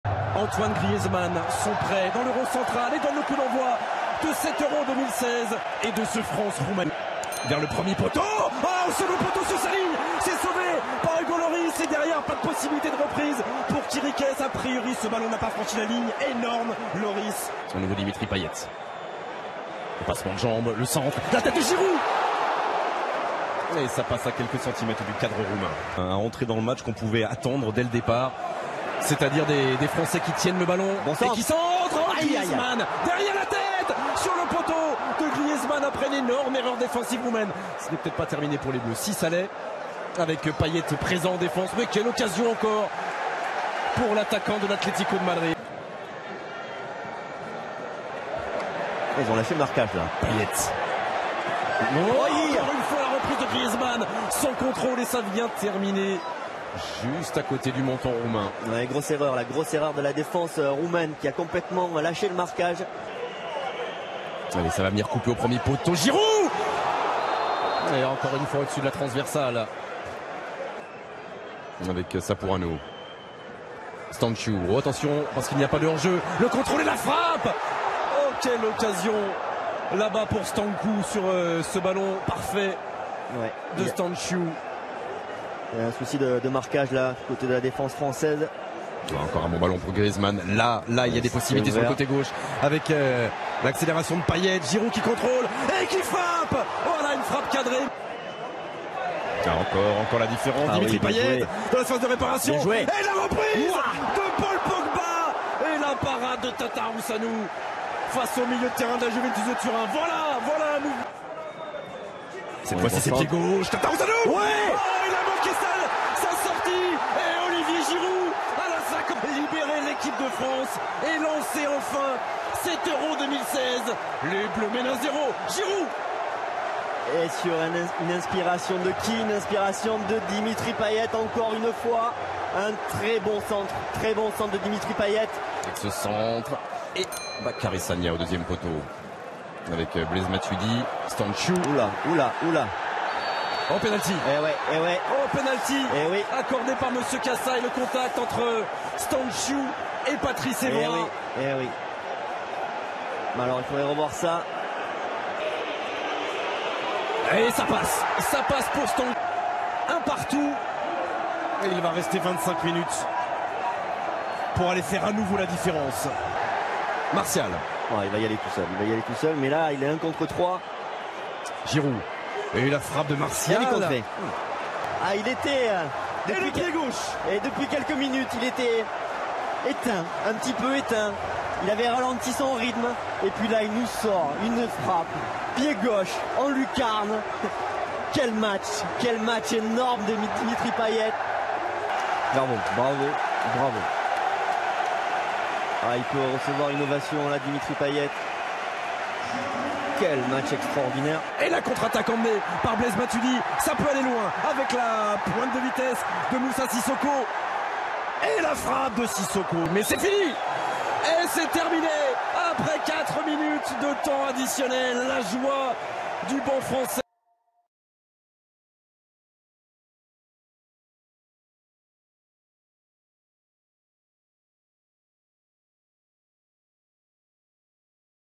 France 2-1 Roumanie Euro 2016 Résumé en Français TF1 HD
France vs Roumanie 2-1 phase de groupes euro 2016 au stade de France a Saint-Denis. but de Giroud et Payet vs Roumanie.